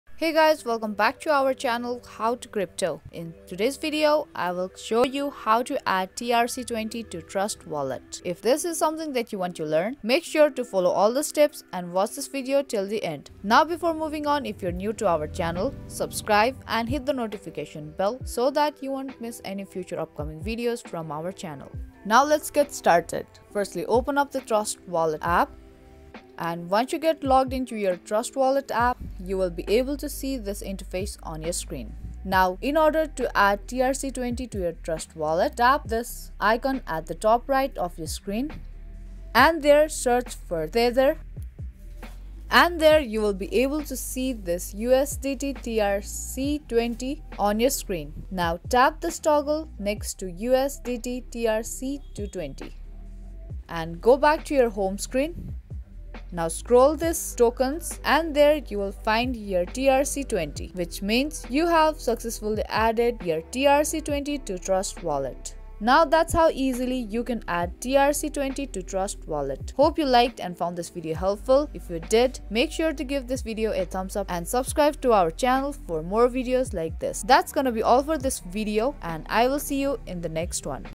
How to Add TRC20 Tokens to Trust Wallet 2023?
In this video, we'll show you how to add TRC20 tokens to your Trust Wallet so you can easily manage and store them. Trust Wallet is a secure and user-friendly crypto wallet that supports a wide range of cryptocurrencies, including TRC20 tokens. Follow these easy steps to add your TRC20 tokens to Trust Wallet.